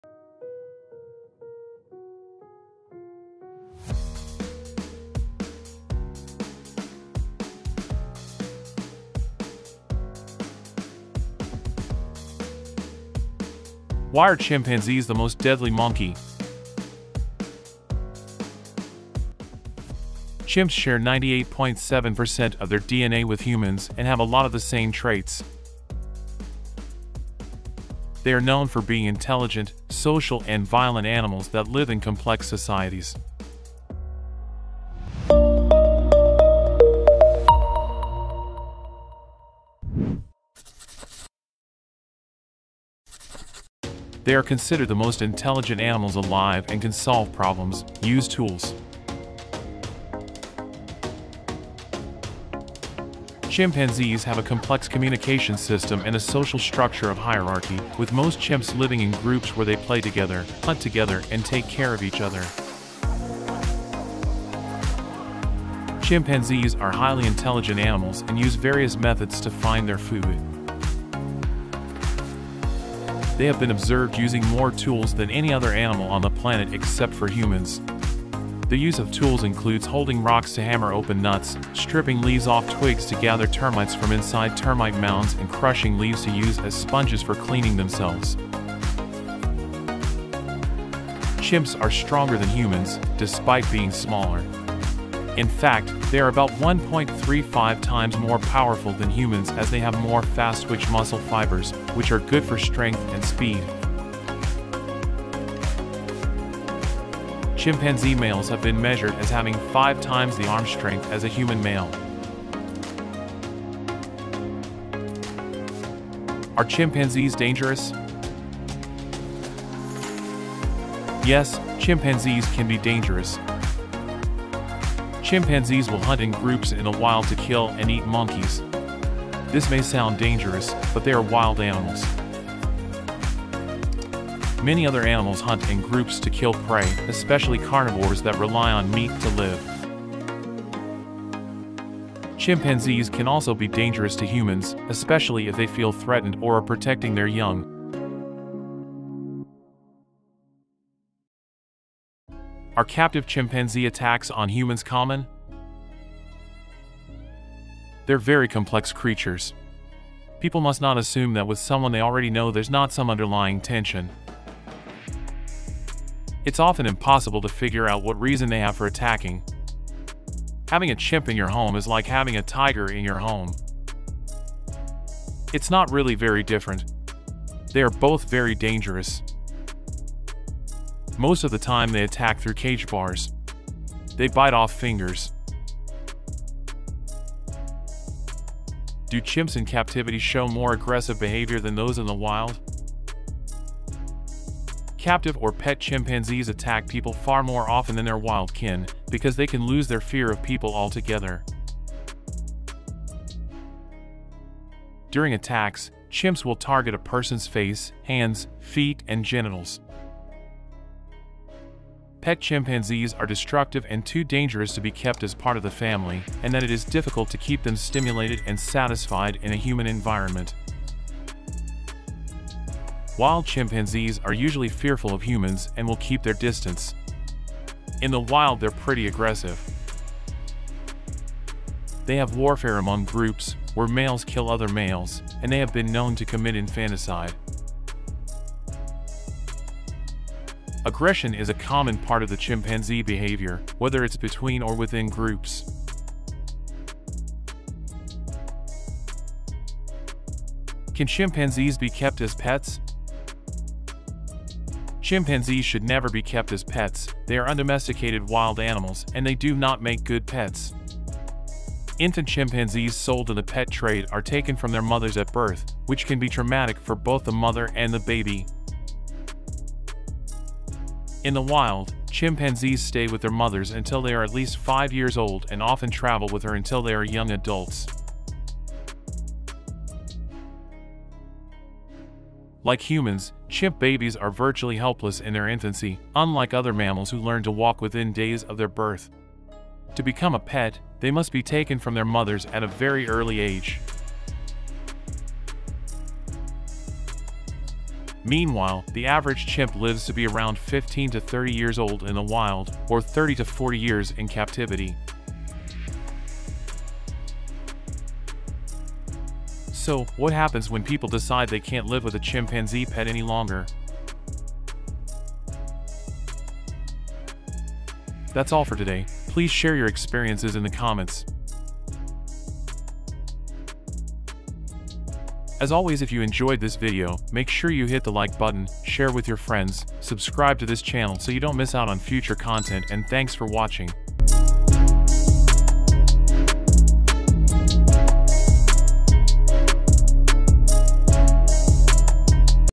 Why are Chimpanzees The Most Deadly Monkey? 🐵 🙈 Can Chimpanzees Be Kept as Pets?
Why are Chimpanzees The Most Deadly Monkey?
Are Chimpanzees Dangerous?

Chimps are stronger than humans, despite being smaller. In fact, they are about 1.35 times more powerful than humans as they have more fast-twitch muscle fibers, which are good for strength and speed.

Recommended Product